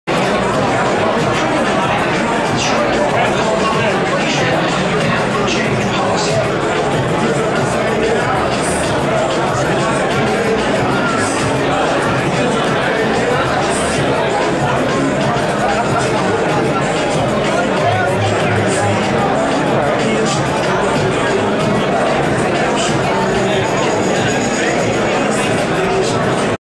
Chicago Ron Paul Rally Part 1
The Ron Paul Rally in Downtown Chicago. An excited crowd welcoming the freedom message of Ron Paul